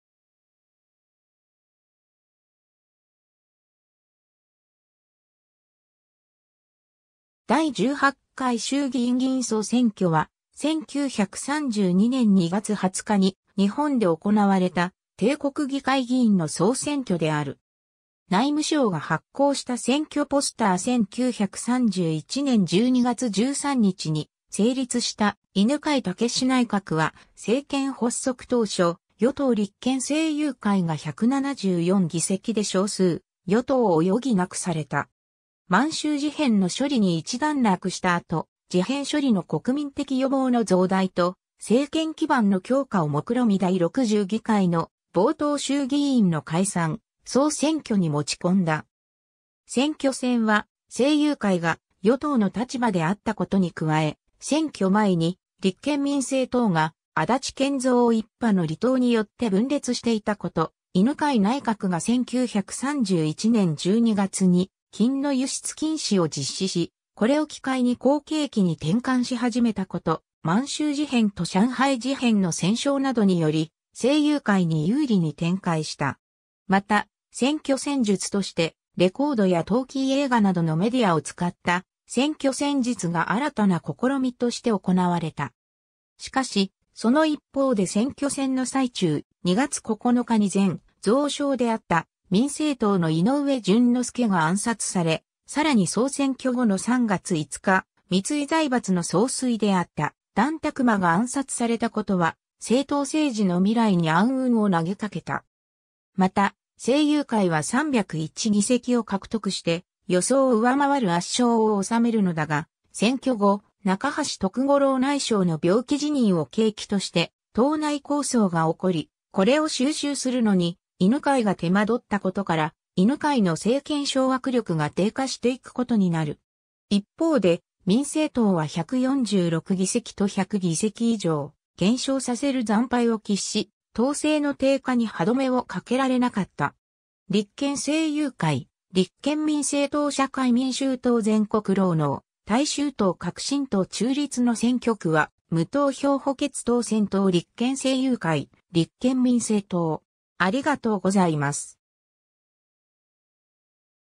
第18回衆議院議員総選挙
第18回衆議院議員総選挙（だい18かいしゅうぎいんぎいんそうせんきょ）は、1932年（昭和7年）2月20日に日本で行われた帝国議会（衆議院）議員の総選挙である。
 内務省が発行した選挙ポスター 1931年（昭和6年）12月13日に成立した犬養毅内閣は、政権発足当初、与党立憲政友会が174議席で少数与党を余儀なくされた。
満州事変の処理に一段落した後、事変処理の国民的輿望の増大と、政権基盤の強化を目論み第60議会の冒頭衆議院の解散・総選挙に持ち込んだ。
 選挙戦は、政友会が与党の立場であったことに加え、選挙前に立憲民政党が安達謙蔵一派の離党によって分裂していたこと（安達一派は選挙後の12月に国民同盟を結成）、犬養内閣が1931年12月に金の輸出禁止を実施し、これを機会に好景気に転換し始めたこと、満州事変と上海事変の戦勝などにより、政友会に有利に展開した。
 また、選挙戦術として、レコードやトーキー映画などのメディアを使った選挙戦術が新たな試みとして行われた。
 しかし、その一方で選挙戦の最中、2月9日に前蔵相であった民政党の井上準之助が暗殺され、さらに総選挙後の3月5日三井財閥の総帥であった團琢磨が暗殺された（「血盟団事件」）ことは政党政治の未来に暗雲を投げかけた。
 また、政友会は301議席を獲得して予想を上回る圧勝を収めるのだが、選挙後、中橋徳五郎内相の病気辞任を契機として党内抗争が起こり、これを収拾するのに犬養が手間取ったことから犬養の政権掌握力が低下していくことになる。
 一方で、民政党は146議席と100議席以上減少させる惨敗を喫し、党勢の低下に歯止めをかけられなかった。
 立憲政友会 立憲民政党 社会民衆党 全国労農大衆党 革新党 中立 *の選挙区は無投票 補欠当選等 立憲政友会 立憲民政党